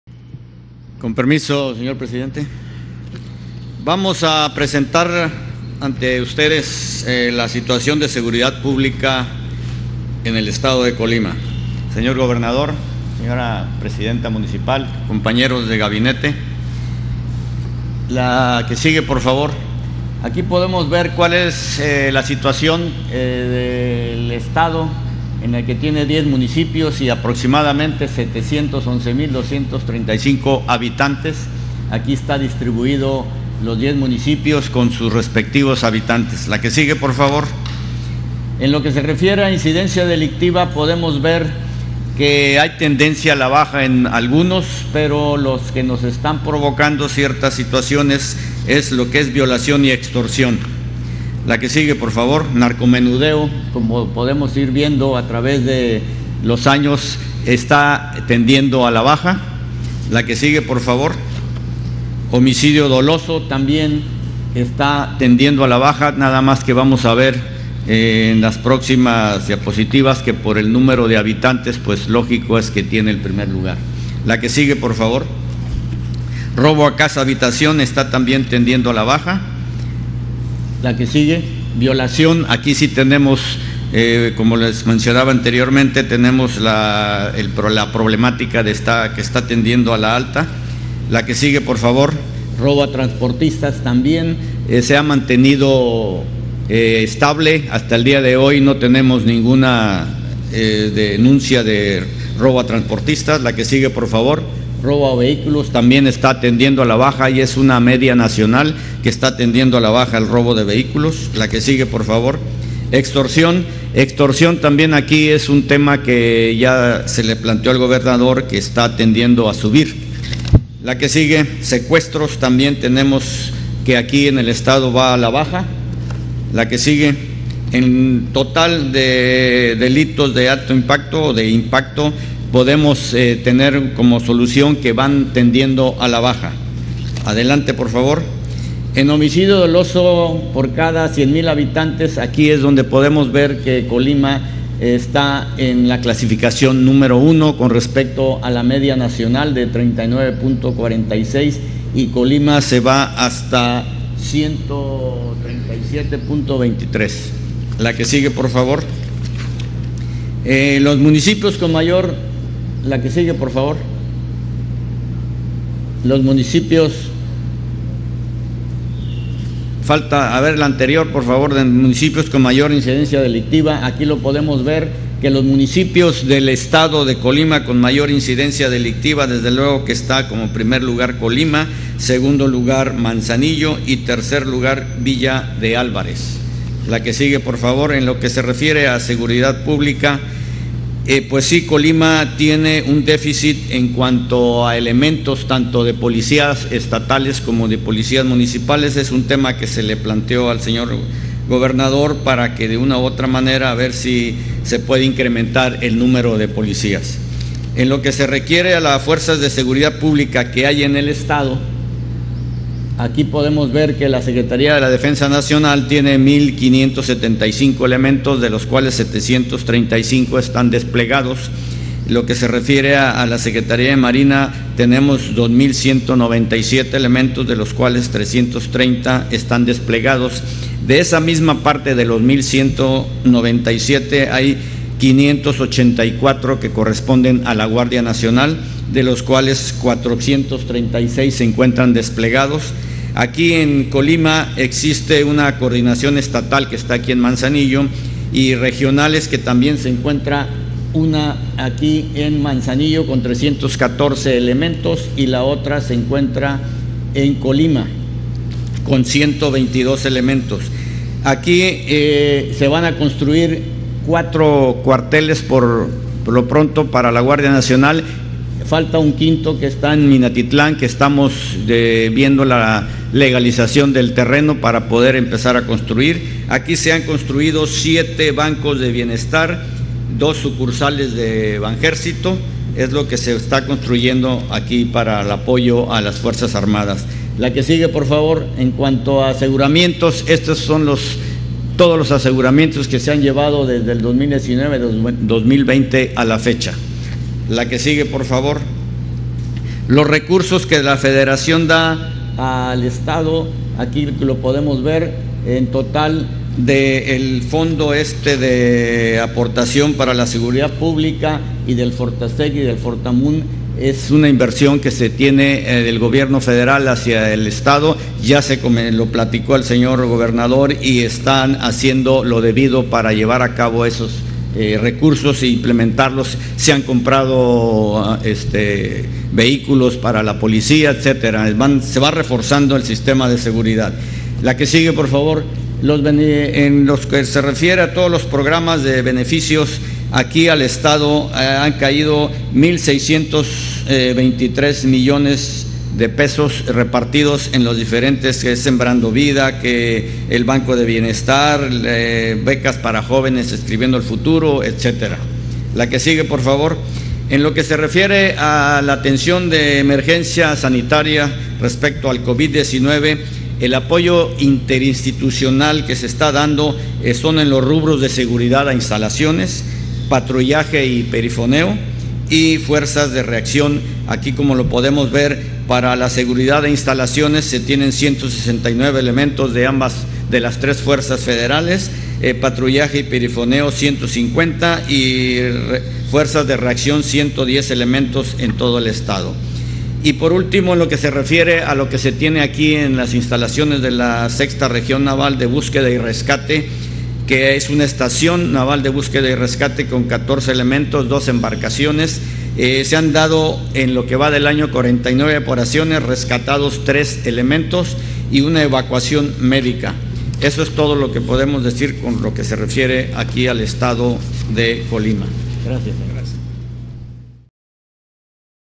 Situación seguridad pública Colima. Almte. Ojeda Durán.17 julio 2020.
Almirante Rafael Ojeda Durán, Secretario de Marina,
Conferencia Presidente Andrés Manuel López Obrador.
Gobierno de México.17 de julio de 2020